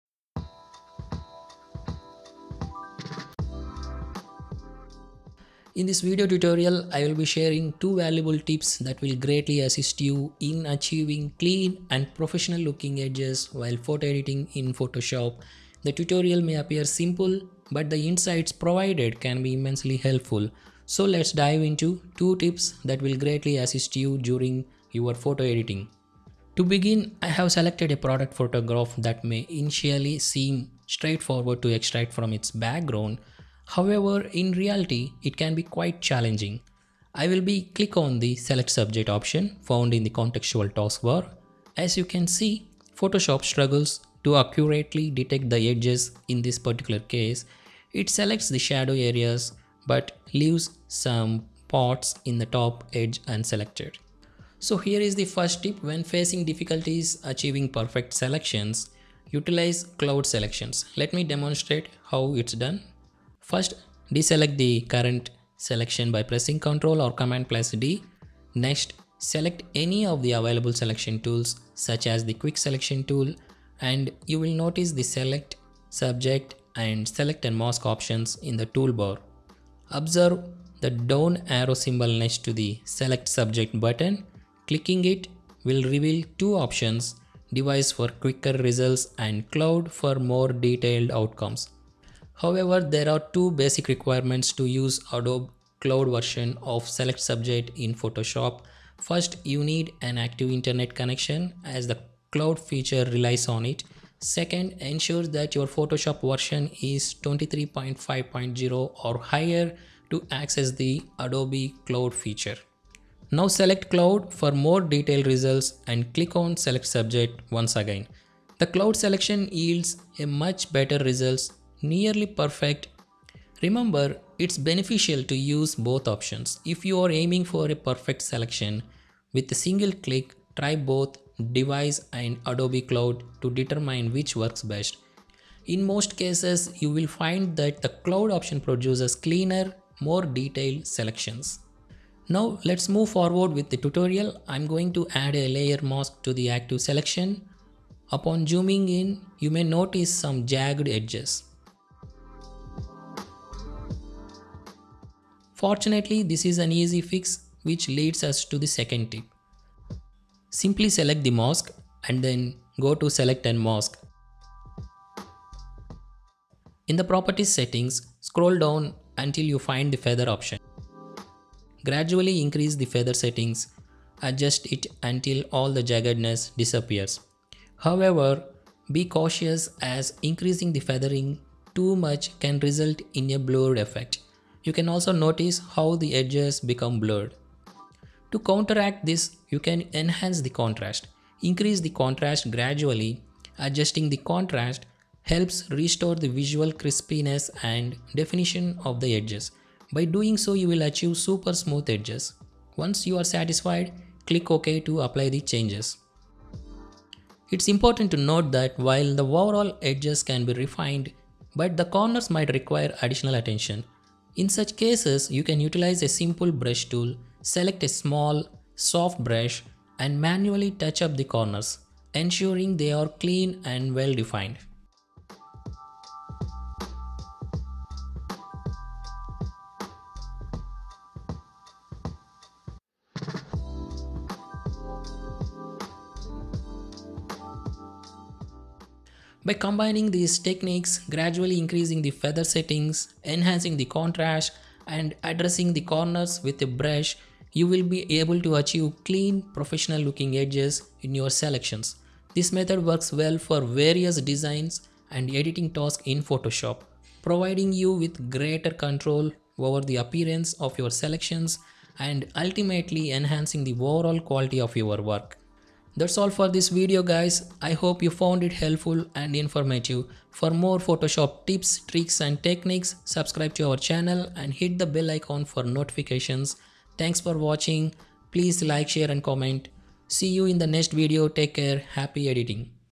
2 Tips for CLEAN & PROFESSION Looking Edges in Photoshop!
In this photoshop photography tutorial, I'll share two valuable tips for achieving clean and professional-looking edges in Photoshop. The first tip involves using cloud selections when facing difficulties with perfect selections. By selecting the "Cloud" option in the toolbar, you can get more detailed results, often yielding cleaner and more accurate selections. Remember to try both the "Device" and "Adobe Cloud" options to determine which works best for your specific case.

Moving on to the second tip, I'll show you how to fix jagged edges in your selections. By adding a layer mask and going to the "Select and Mask" settings, you can gradually increase the feather setting to eliminate jaggedness. Be cautious not to overdo it, as excessive feathering can result in a blurred effect. To counteract this, you can enhance the contrast gradually, which restores crispness and definition to the edges. Finally, manually touch up the corners with a small, soft brush to ensure they are clean and well-defined.

By combining these techniques, you can achieve clean and professional-looking edges in your selections, enhancing the overall quality of your work.